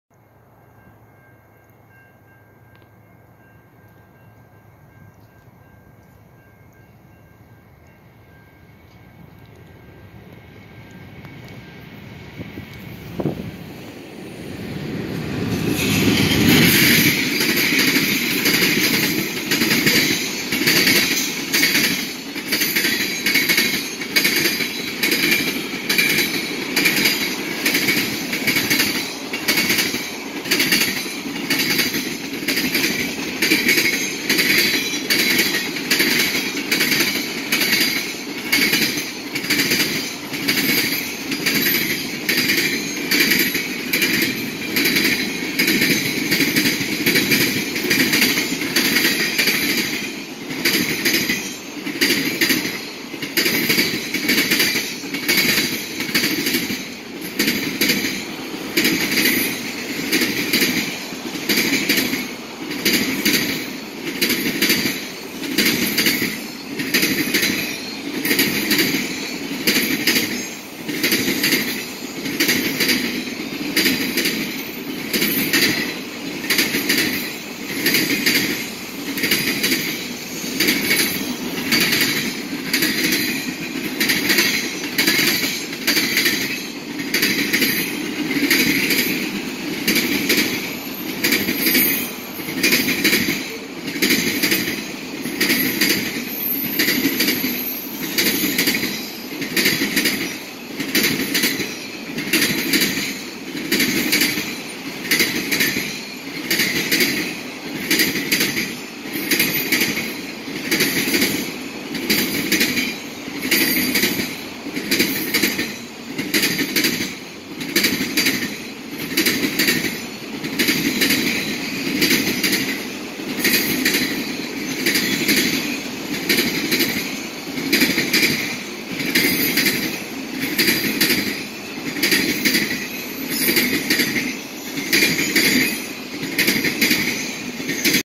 CN with 2 out of 3 units are cowls
Westbound at CN Carew this CN freight has 2 cowl units in 2 different paint schemes. It's getting rare to see 2 days before 2020 arrives